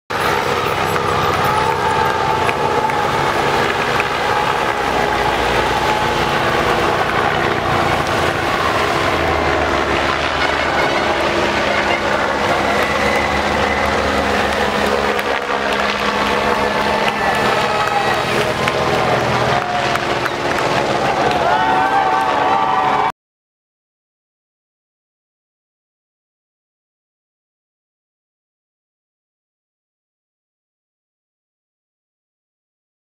Salto sobre o Tour de France 2013
Roman Marandet, a equipe EnchoRage , ajudou Kevin Demolis Alexis Bosson, Killian Bron e Emeric Caron a saltar sobre a etapa do Tour de France em 2013, neste sábado, 20 de julho. Cuidado com a cabeça!